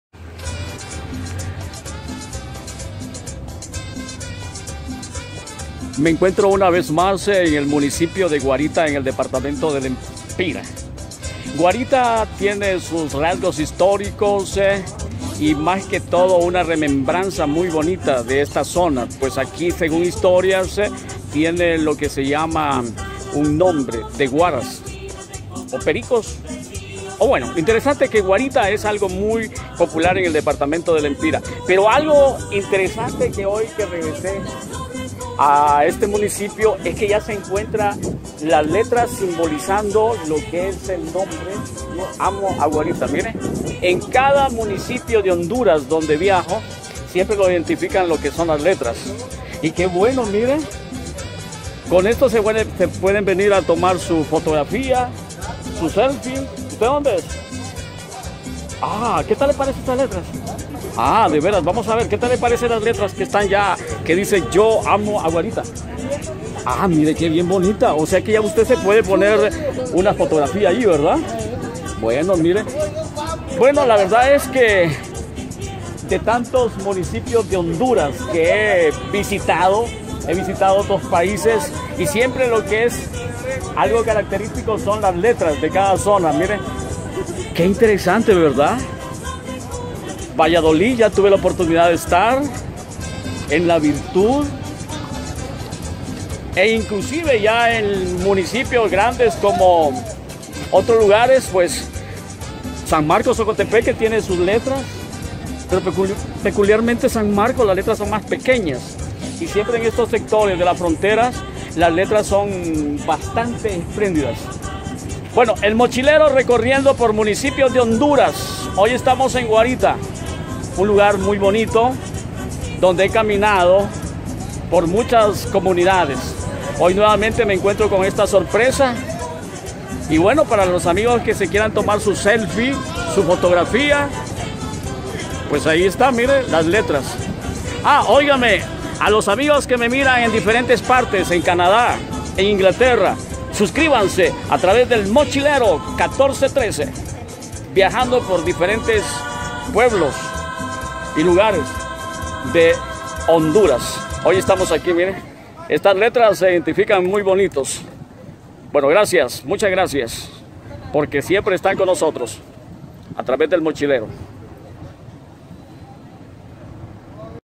EL MUNICIPIO DE GUARITA LEMPIRA YA CUENTA CON LAS LETRAS REPRECENTATIVAS A EL MUNICIPIO
HACE ALGUNOS DIAS EL MUNICIPIO DE GUARITA YA CUENTA CON LAS LETRAS ALUCIVAS DEL MUNICIPIO